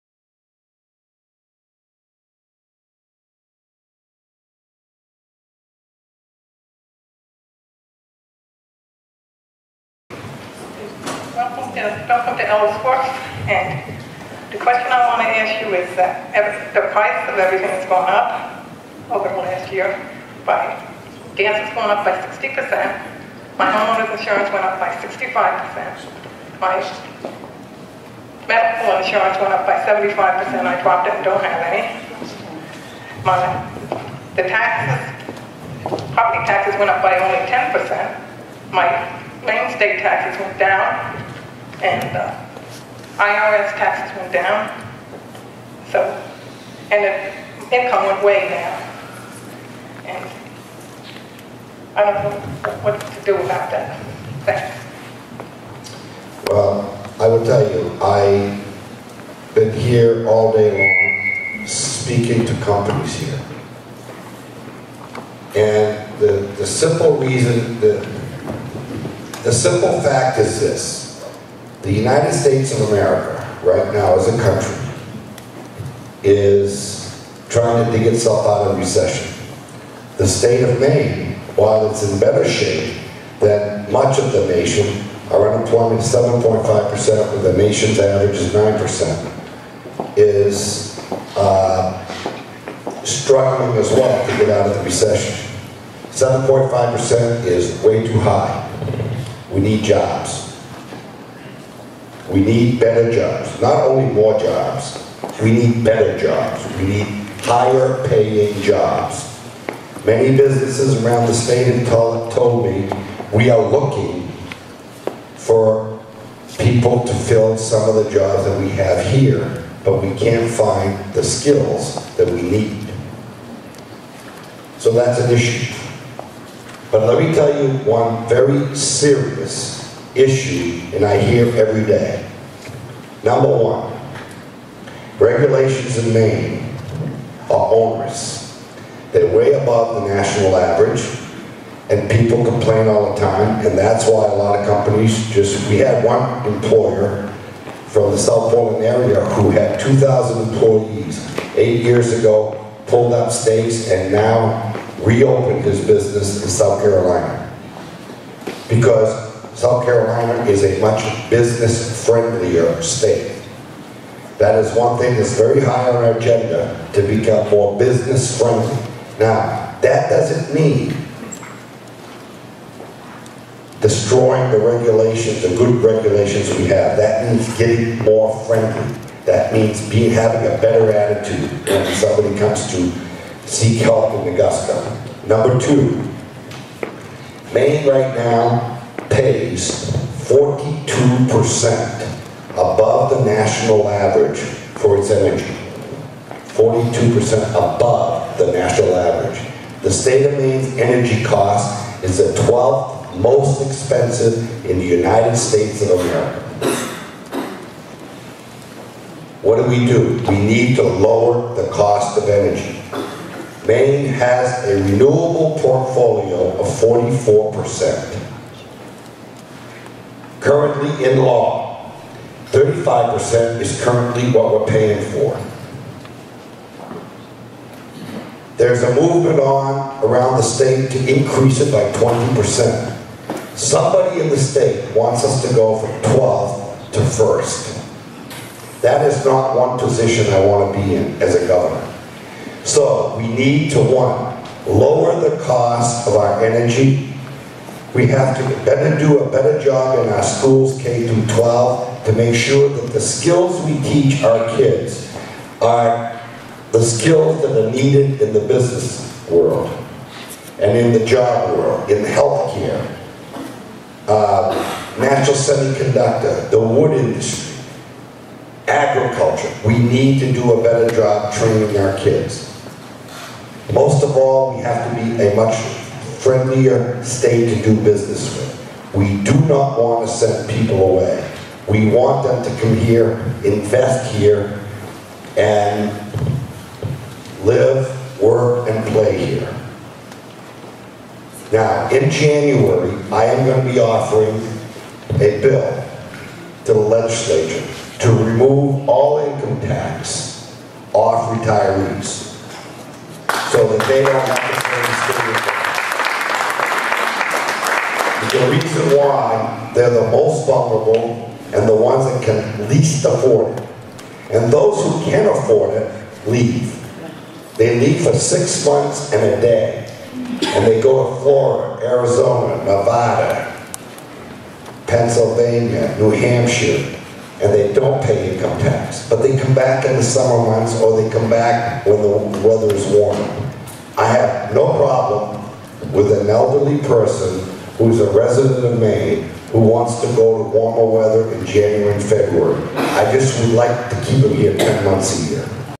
Maine Gov. Paul LePage on the need for better jobs
Gov. Paul LePage held a Town Hall meeting in Ellsworth, Maine, on 10 November 2011. At it LePage was asked about how to improve the economy in the Pine Tree State.